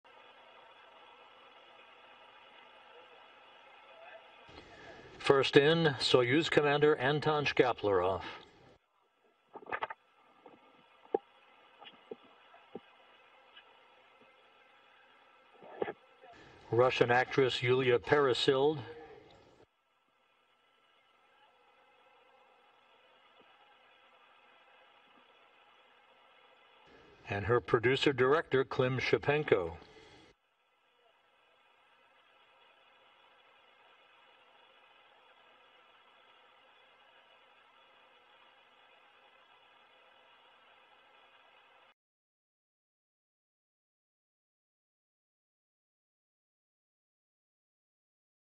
WEB EXTRA: Russian Actress, Director Arrive at International Space Station to Film Movie
A Russian actress and a director arrived at the International Space Station on Tuesday Actress Yulia Peresild and director Klim Shipenko will spend 12 days aboard the ISS to shoot part of a movie about a medical emergency in space.